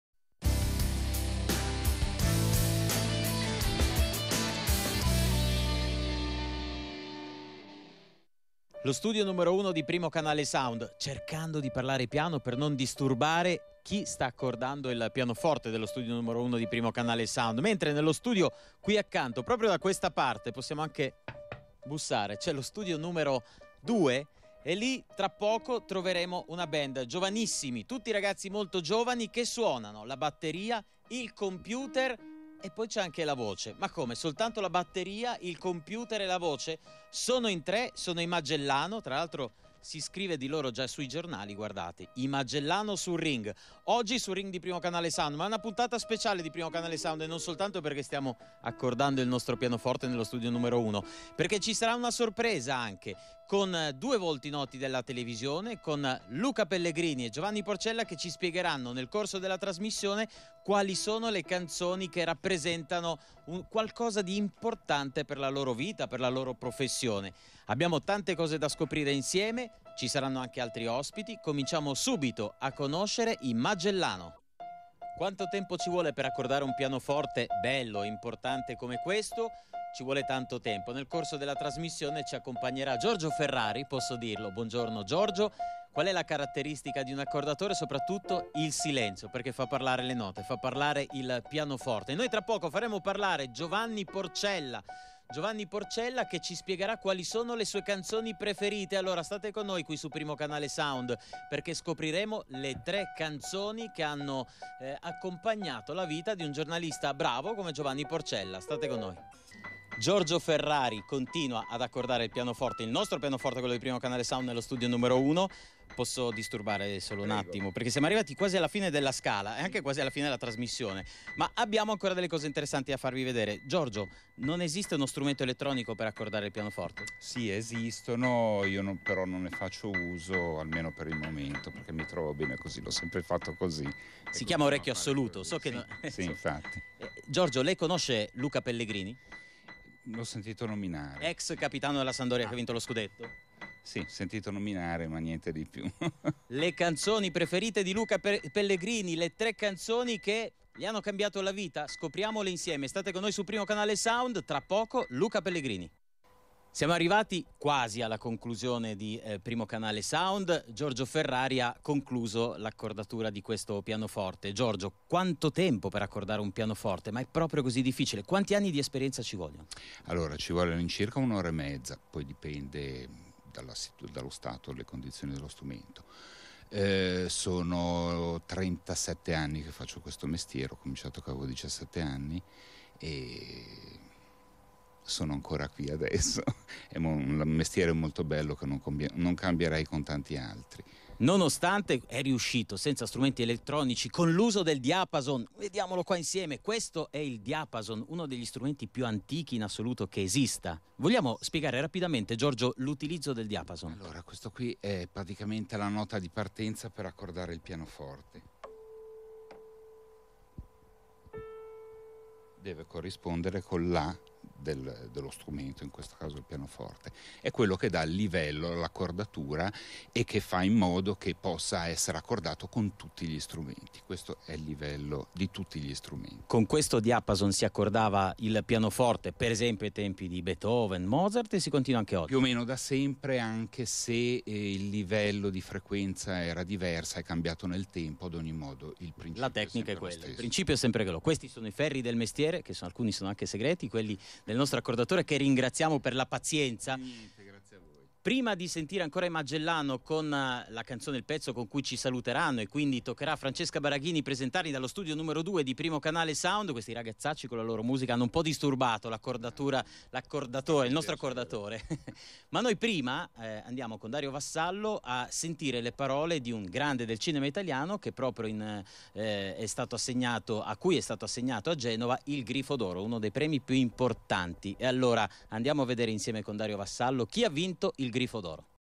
Giorgio Ferrari accordatore
Giorgio Ferrari accorda il pianoforte di Primocanale Sound